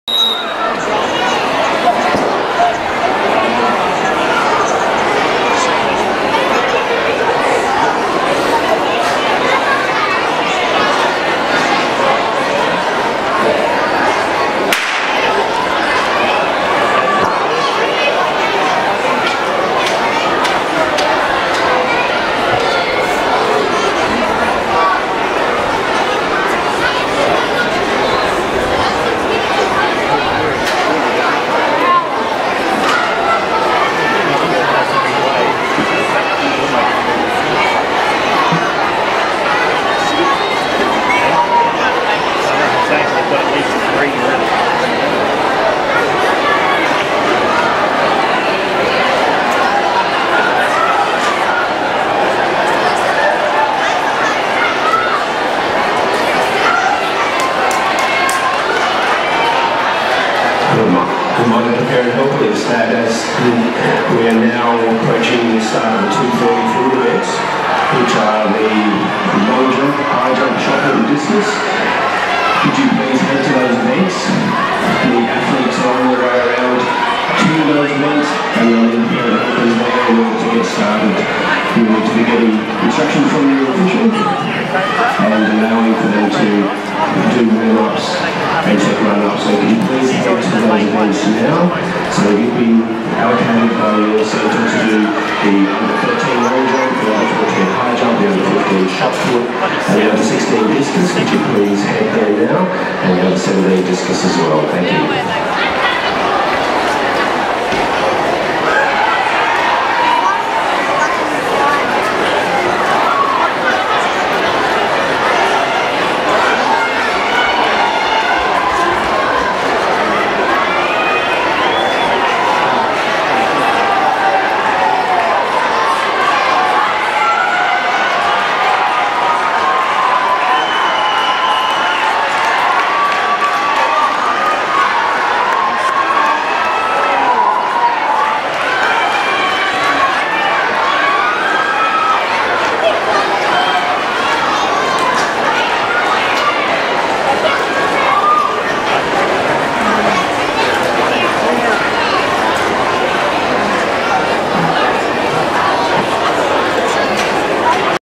Jordan McMillan LAA State Relay Day u13 4x200m Final 2017
The Girls nearly didn't get the baton around. Once again the race was over before Jordi gets to run.